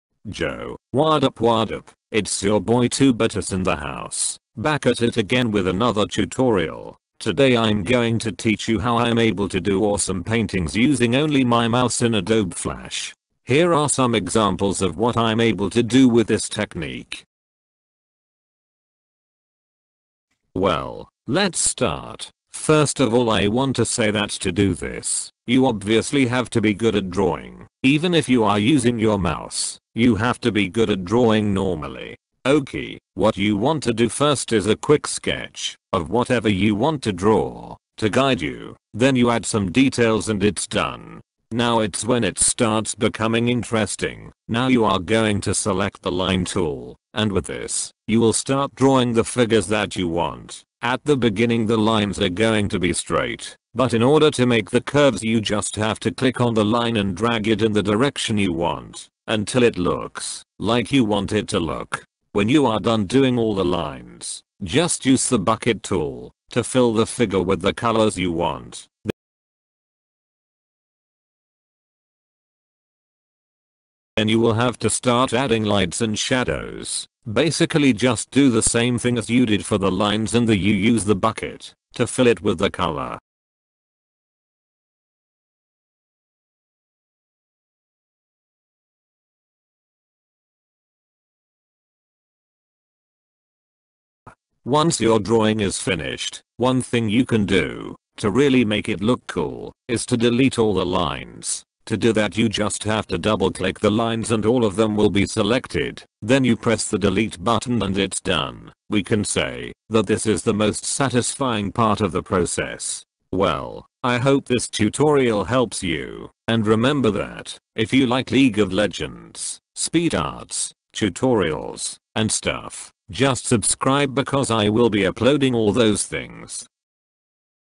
How to draw and paint using a mouse in Adobe Flash tutorial
In this tutorial I will teach you how to draw or paint in adobe flash using only your mouse, without any graphic tablet like wacom tablets or anything